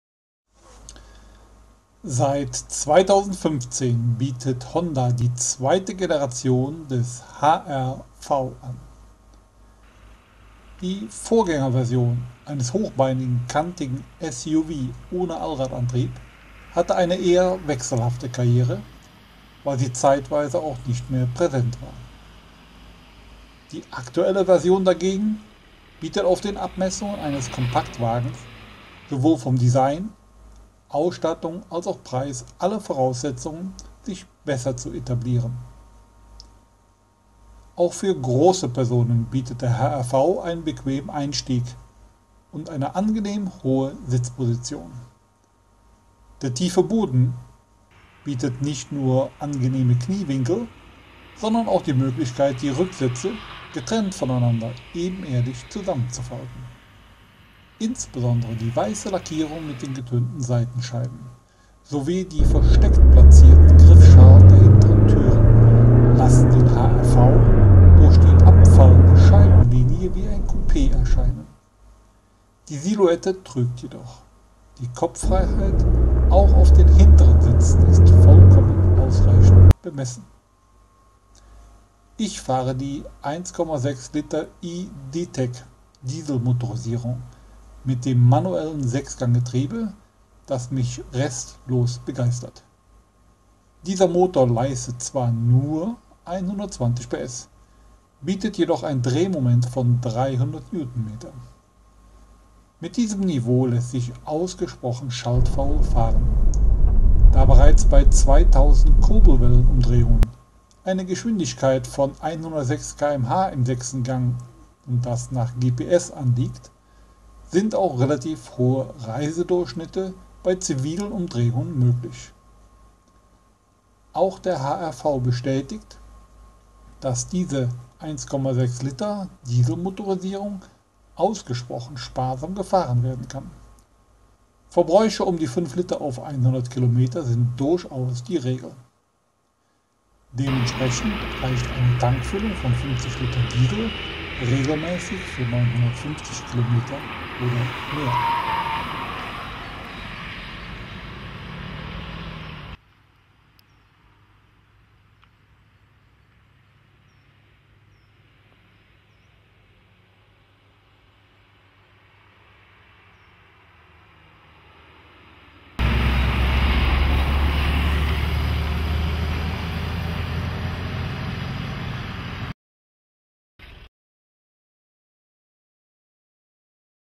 Onlinemotor Honda HR-V 1.6 i DTEC 88kW120PS
Auch wenn der HR-V von Honda in Deutschland eine eher wechselhafte Karriere begonnen hat, hat die seit 2015 angebotene 2. Generation jegliches Schattendasein beendet. Ich glaube, dass sich ein genauerer Blick durchaus lohnen könnte. Mit einer Länge von 4.294mm entspricht der HR-V exakt den Kompaktmaßen. Er ist mit einer Höhe von 1.605mm etwas überdurchschnittlich und bietet damit mehr Einstiegsqualitäten. Da die Türgriffe der hintern Türen im Bereich der abgedunkelten Scheiben platziert sind fällt auch der bequeme Zustieg auf die hinteren Plätze erst auf den 2. Blick ins Auge. Die abfallende Scheibenlinie suggeriert ein abfallendes Coupè-Heck, was jedoch gerade in der weißen Lackierung täuscht. Tatsächlich bleibt die Dachlinie nahezu waagerecht und bietet damit auch ausreichenden Kopffreiheit für die rückwärtigen Passagiere. Der HR-V hat einen sehr tiefen Fahrzeugboden. Das kommt dem Sitzkomfort insbesondere der rückwärtigen Passagiere entgegen und bietet die Möglichkeit die Rücksitze ebenerdig einzufalten. Comfort, Elegance und Executive steigern sich die Ausstattungslinien. Den City-Notbremsassistenten bietet bereits ein Einstiegsversion. Das Kollisionswarnsystem, Spurhaltesystem und die Verkehrszeichenerkennung starten mit der Ausstattung Elegance. Die Rückfahrkamera ist Bestandteil der von mir gefahrenen Ausstattung Executive. Da der HR-V sehr beachtliche Reisequalitäten mit sich bringt habe ich mich auch von dem 7Zoll großen Touchscreen mit schneller Garmin Software bestücktem Navi überzeugen lassen. Ich bin den 1.6i-DTEC mit 88kW/120 PS nicht zu ersten Mal gefahren. Diesen mit 300Nm sehr souveräne ausgestatteten 4 Zylinder Diesel ist lebendig und mehr ausreichend kräftig, um auch sehr schaltfaul durch die Lande zu sparen. Antrieb Die Kombination mit dem manuellen 6-Gang Schaltgetriebe passt perfekt. Nicht nur die Bedienung passt sondern auch die Gangsprünge. Bei 2.000 Kurbelwellenumdrehungen liegt eine Geschwindigkeit von 106km/h (GPS) an. Bei immer noch sehr leisen 2.500min-1 ist die Autobahnrichtgeschwindigkeit bereits überschritten. Eine Tankreichweite (50ltr.) von 900KM sind wirklich regelmäßig erzielbar. Dafür kann sogar die Autobahnetappe auch mal mit 150-160km/h absolviert werden. Auch ohne das Prädikat "Spritsparfuchs" ist ein tatsächlicher Verbrauch von unter 5 ltr/100KM (Nachtankmenge) zu realisieren. Fazit SUV ohne Allradantrieb mit großzügiger Kopffreiheit im Innenraum und knausrigem und gleichzeitig souveränem Diesel.